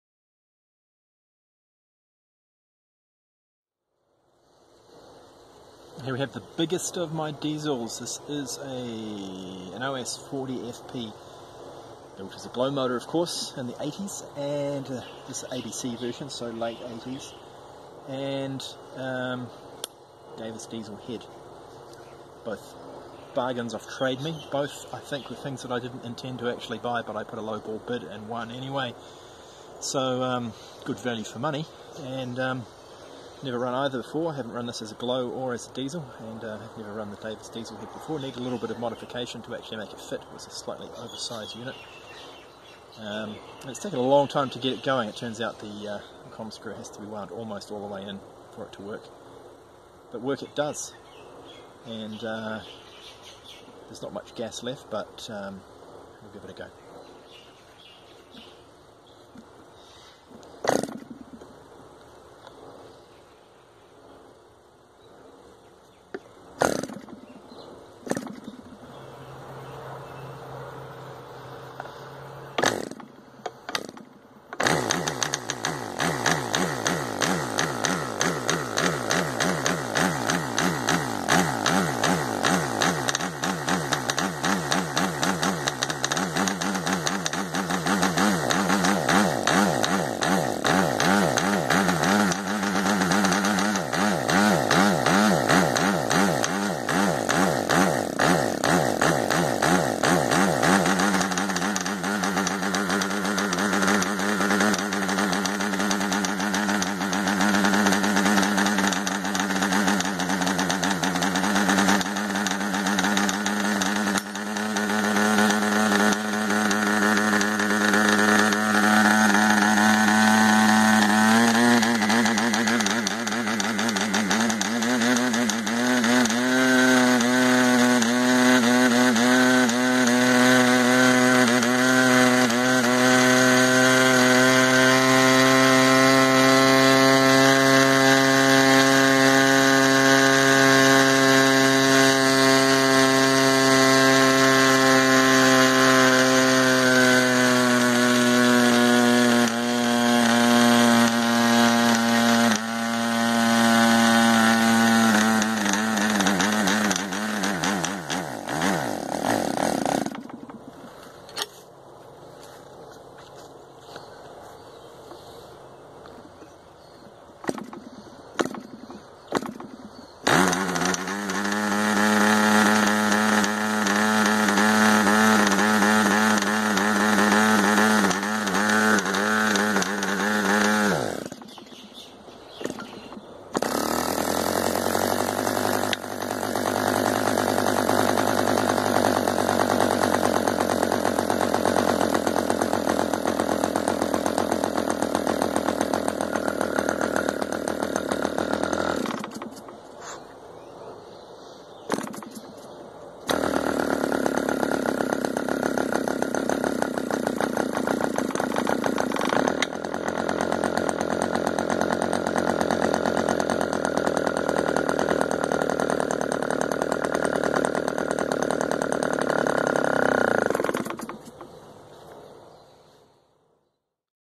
Lockdown Engine Run - OS Max 40FP Diesel
Engine of the day: This 40FP started life as a methanol-breathing glow engine, but now wears a Davis Diesel Developments head and sips kerosene and ether. It's low idle speed and torque are leading me to consider it as the powerplant for a CL scale Typhoon turning a scale 4-bladed propeller.